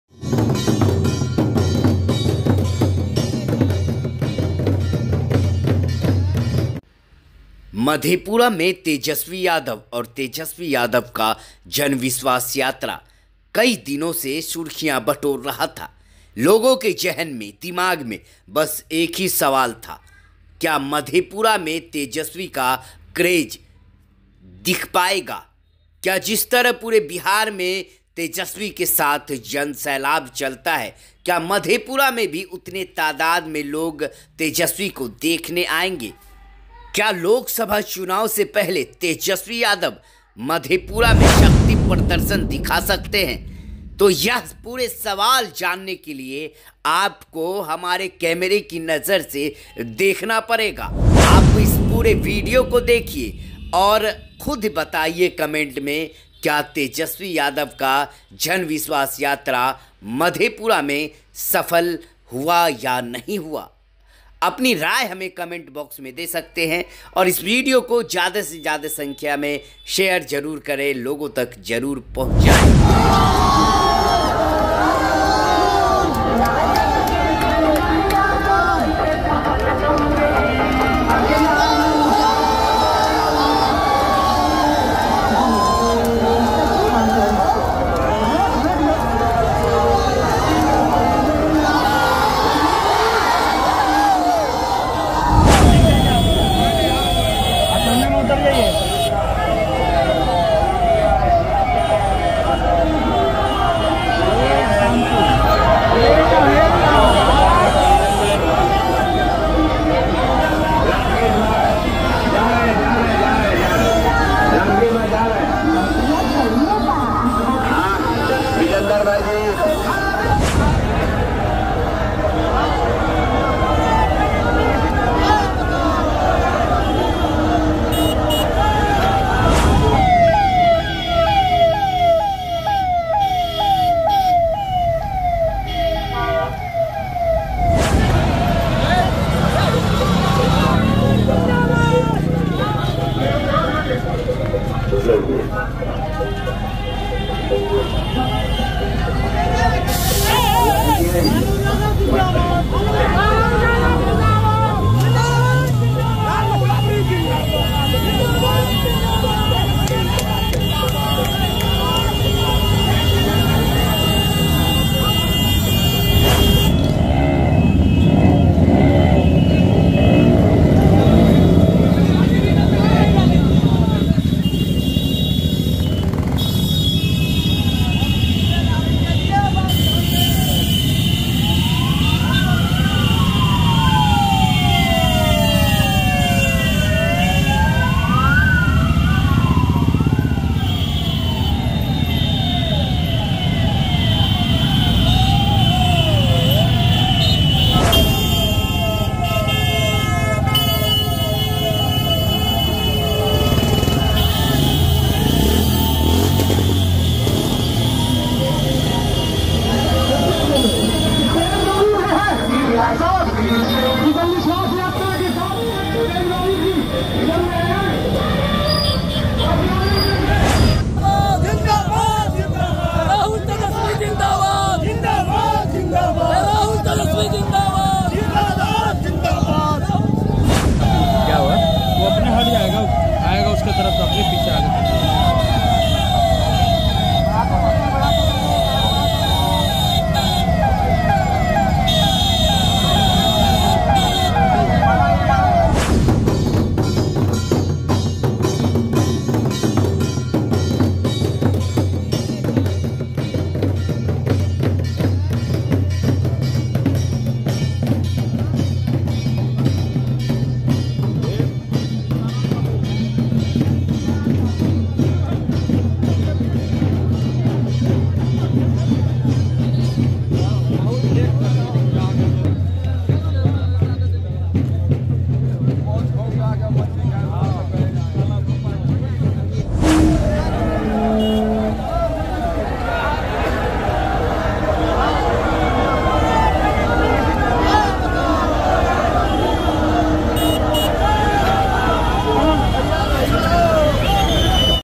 मधेपुरा मे तेजस्वी के जन विश्वास यात्रा मे कितना भीड़ जुटा...?देखिए वीडियो मे..
मधेपुरा मे तेजस्वी के जन विश्वास यात्रा मे कितना भीड़ जुटा...?
देखिए वीडियो मे..